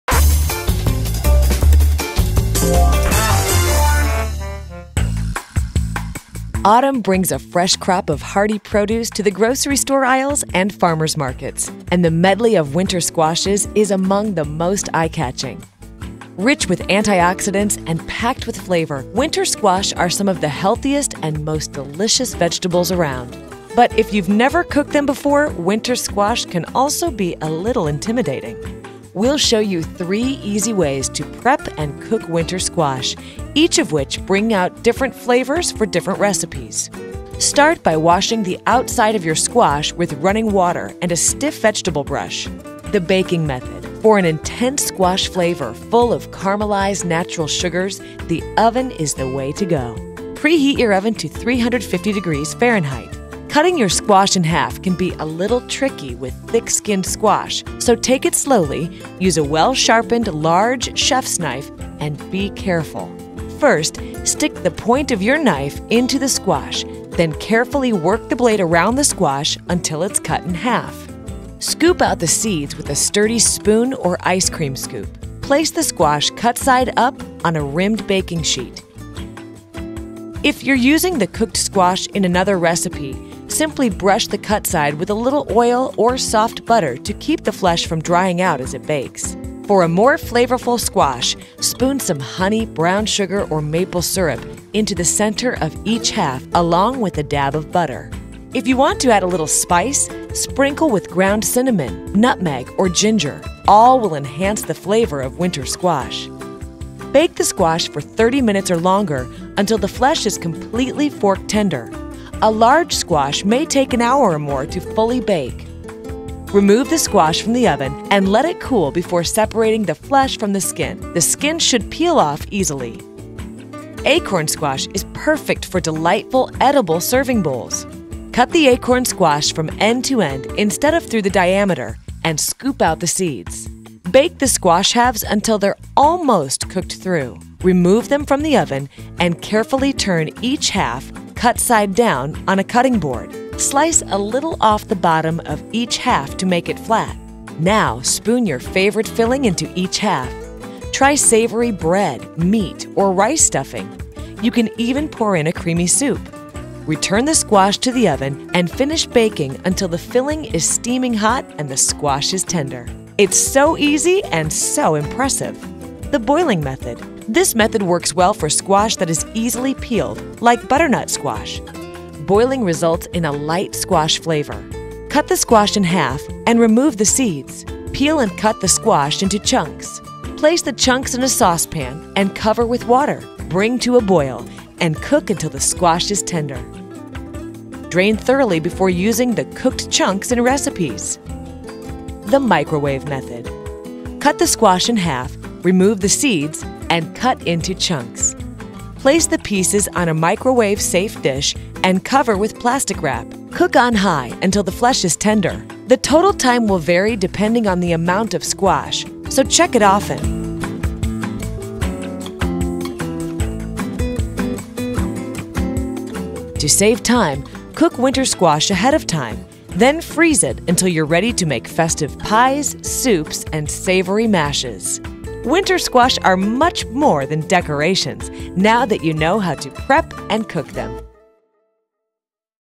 How to Prepare and Cook Winter Squash
Discover different ways to prep and cook winter squash.

Autumn brings a medley of winter squash to grocery stores and farmers' markets. Eye-catching as centerpieces, winter squash (like butternut, pumpkin, and acorn squash) are also some of the healthiest and most delicious vegetables around. In this video, you'll learn how to prep and cook winter squash, each method bringing out different flavors for different recipes from squash casseroles and soups to roasted squash. The first method you'll see is the baked squash method, which builds intense, caramelized flavors. You'll learn tips for keeping the squash from drying out as it bakes and other tips for enhancing flavors. You'll learn how to prepare butternut squash and acorn squash, including a way to turn acorn squash into bowls filled with delicious stuffings. You'll learn the best kind of squash to cook using the boiling method. There's even a method for cooking squash in the microwave. You'll also see a simple technique for cutting thick-skinned squash with a well-sharpened chef's knife.